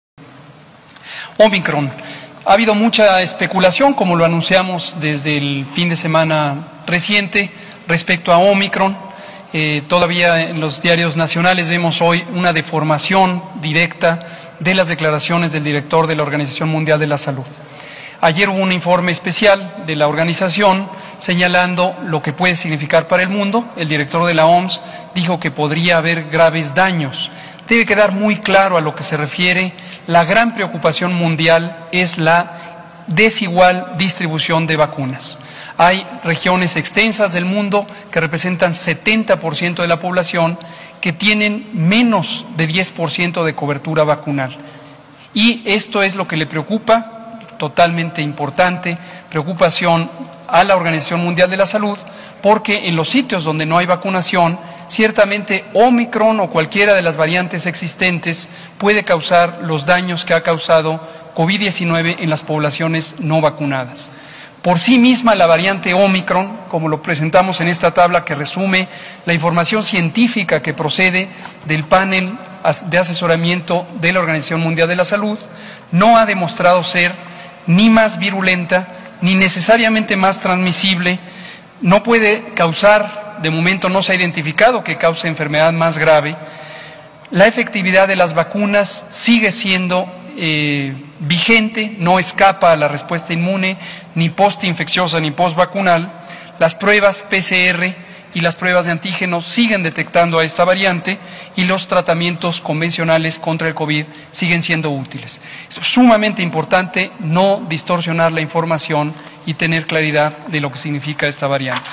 OMS preocupada la distribución desigual de vacunas: Gatell
El Subsecretario de Prevención y Promoción de la Salud, Hugo López-Gatell, señaló que la principal preocupación de la Organización Mundial de la Salud sobre Omicron, es la distribución desigual de vacunas, ya que no se ha demostrado que sea más virulenta.